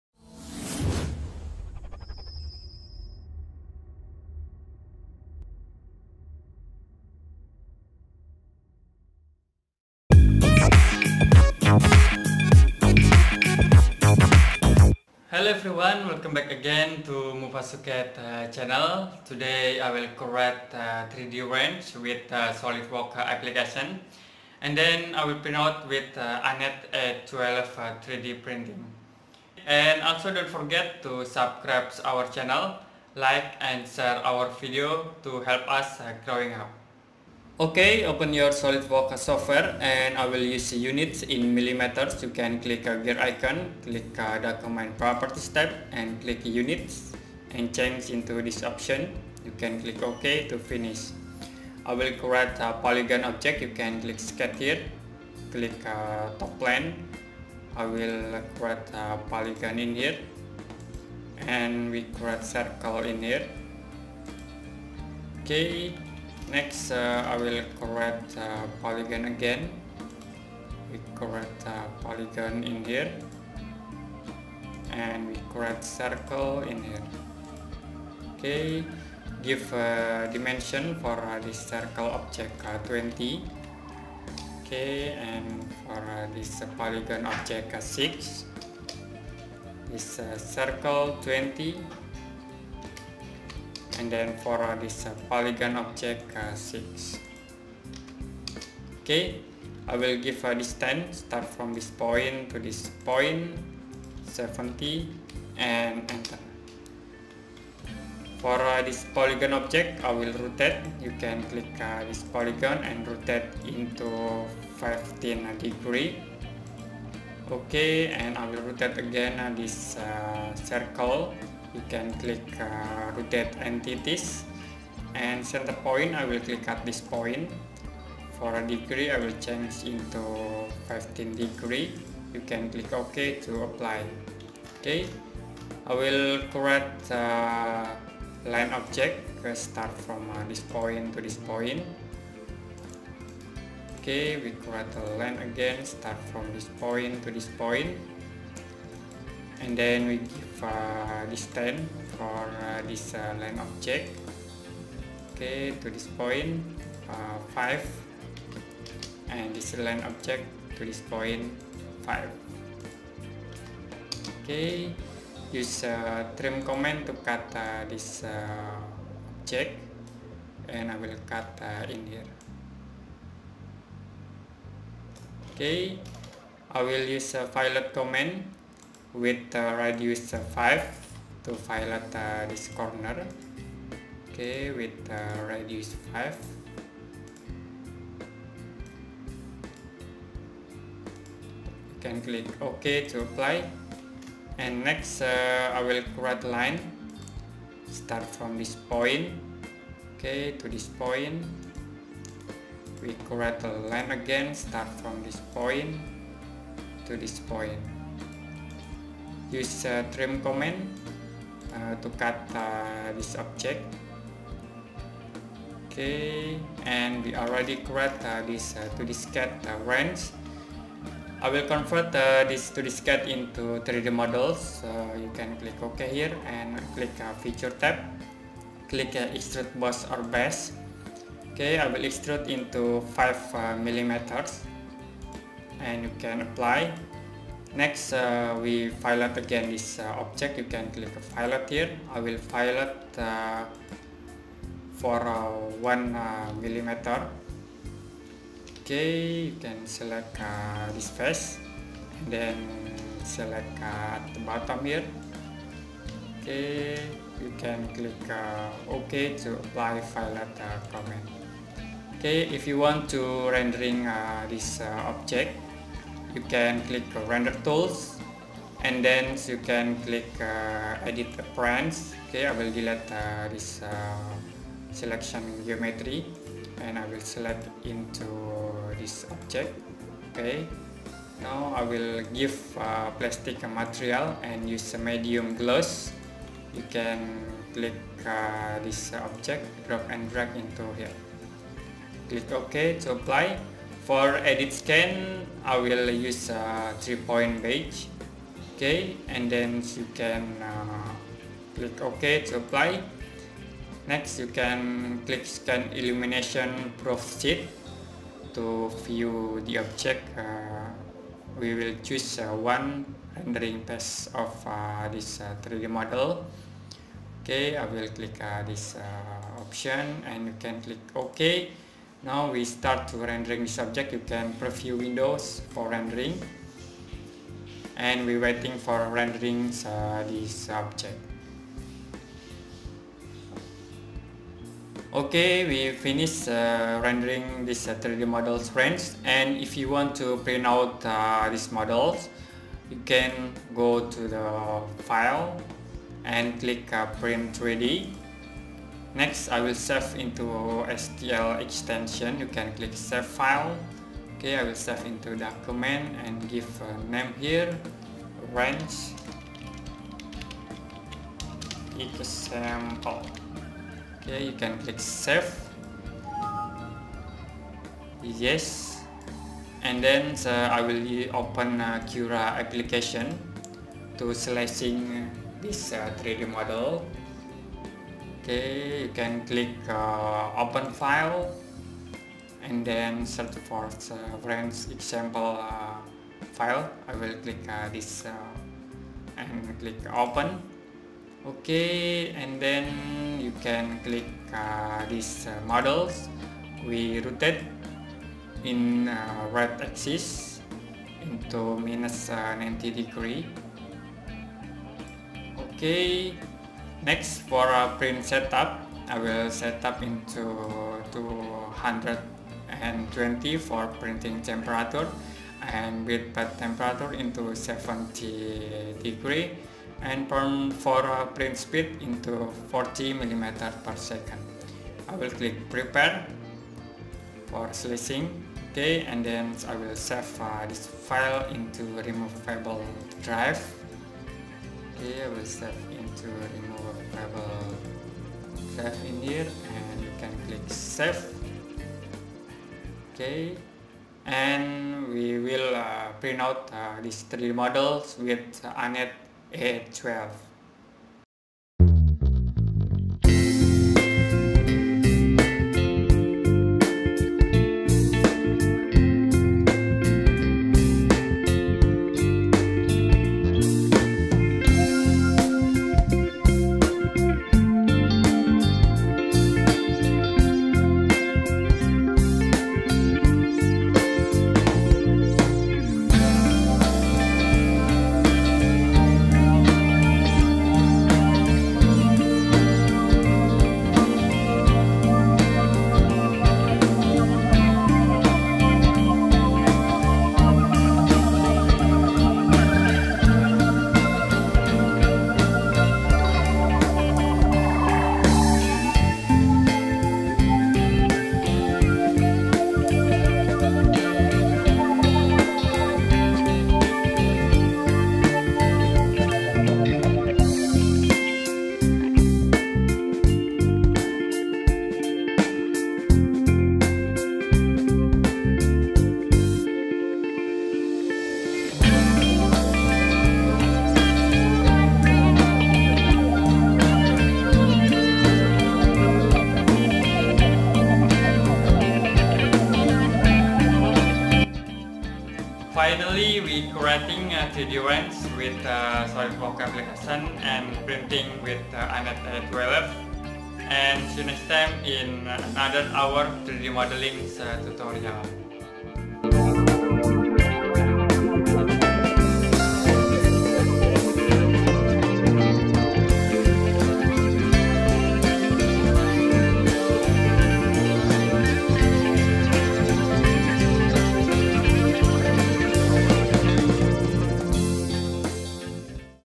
Solidworks 3D Printing Design Wrench With Anet E12 Modeling Tutorial
Learn Solidworks 3d printing tutorial with anet e12 how to make wrench with our drawing exercise practice for beginner at basic level, check it out!!!